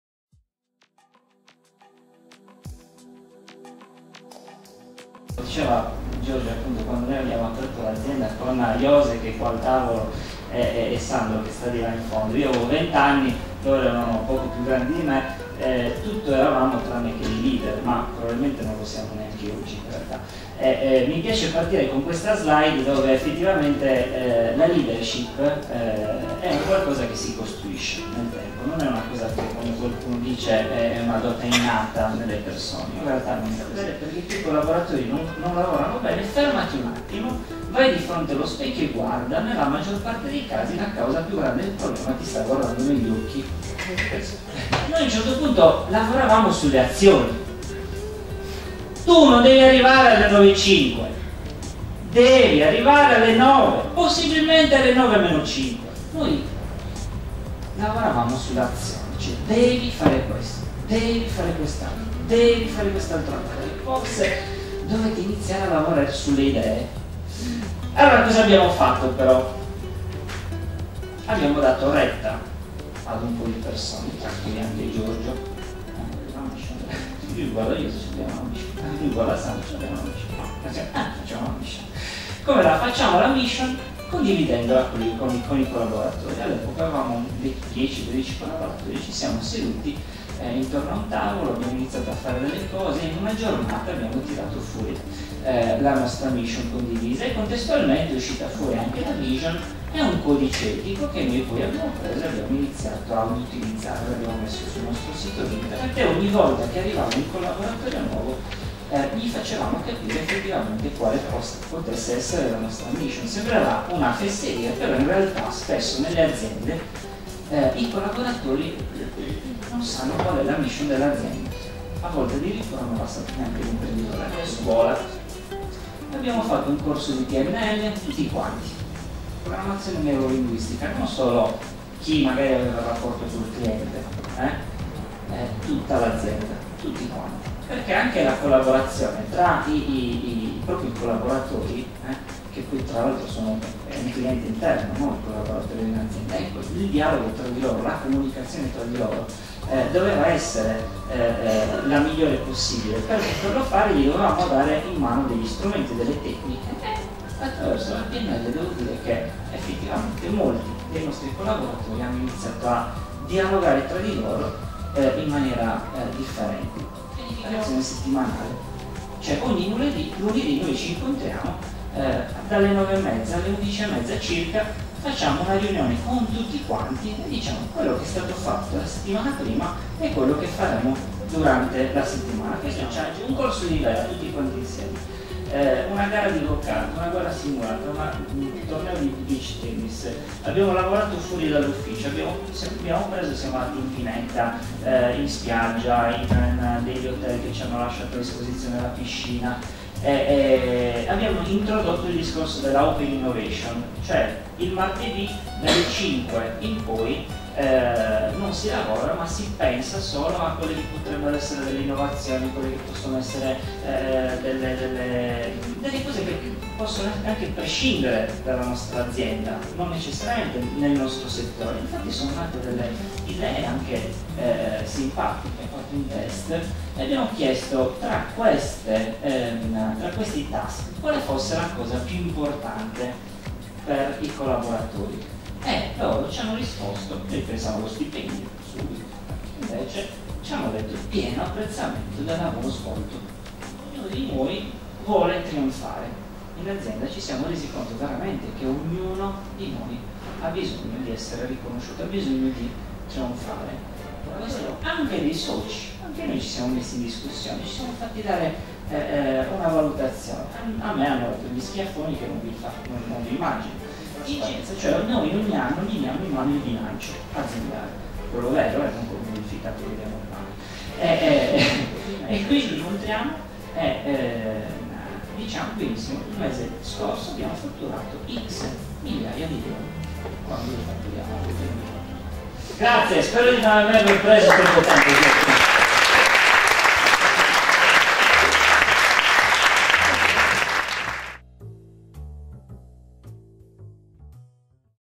Alessandro Vagnozzi al Seminario sulla Leadership. Cagliari 11 aprile 2015
La leadership attraverso la condivisione del senso di appartenenza ed il gioco di squadra